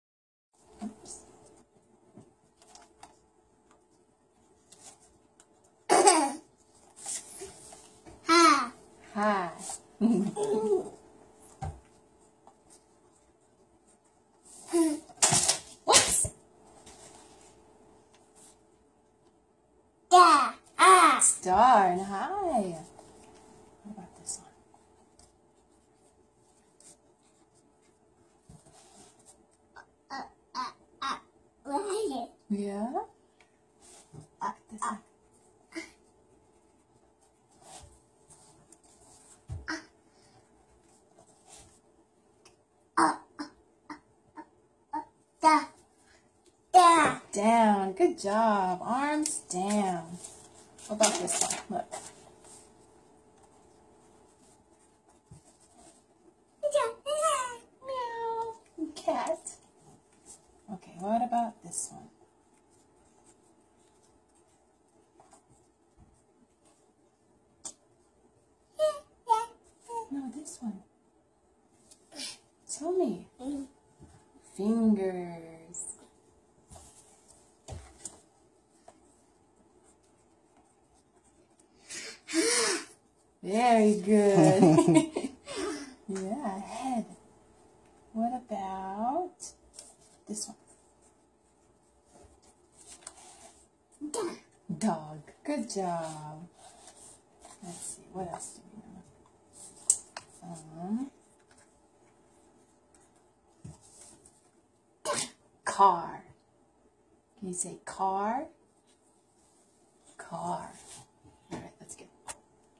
Baby genius reads words
Enoch at 16 months old reading some words